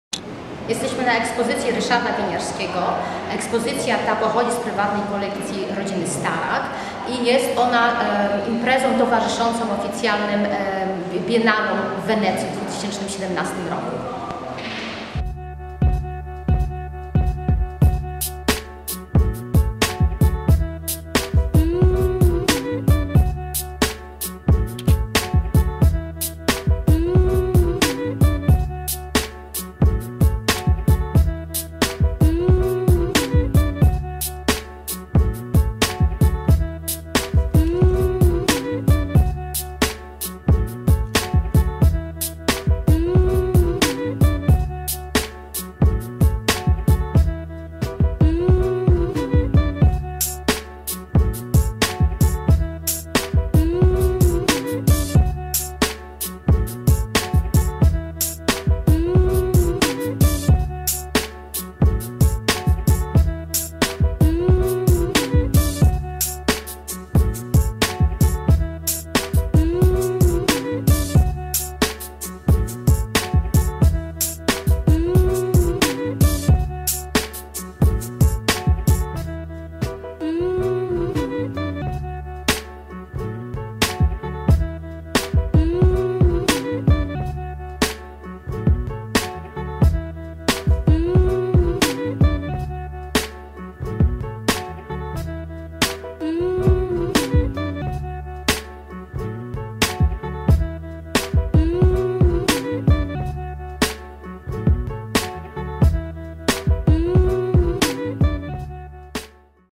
Wystawa Ryszarda Winiarskiego: Biennale 2017
Wystawa Ryszarda Winiarskiego: Biennale 2017, Wenecja. Kolekcja z Fundacji Rodziny Starak.